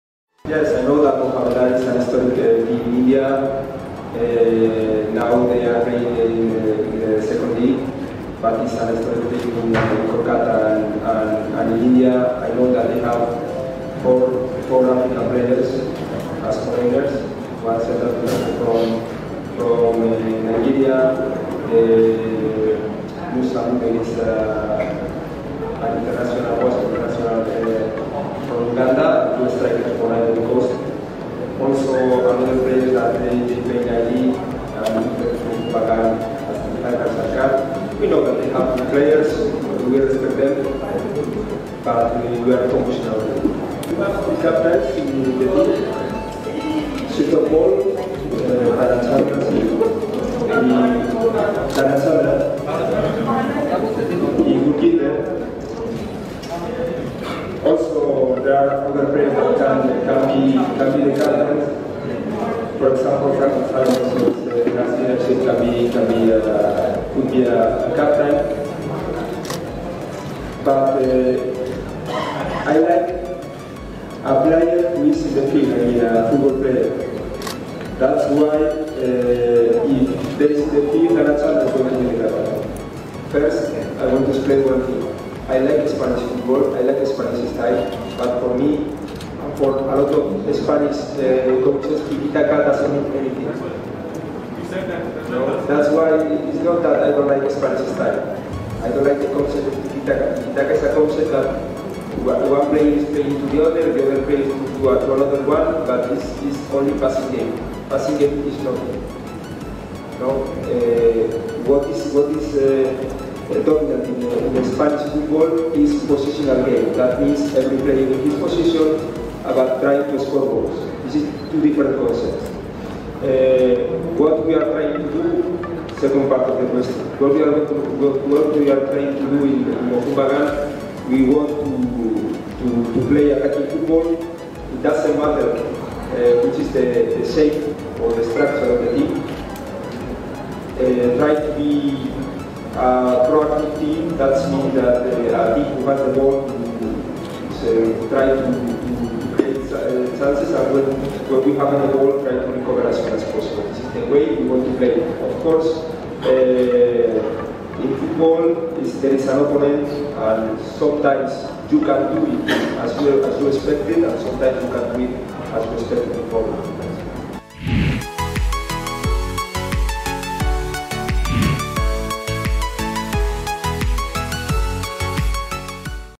Salva Chamorro's post-match conference
Man of the Match Salva Chamorro sharing his thoughts after scoring a match winning brace in his debut for Mohun Bagan in the opening game of Durand Cup 2019.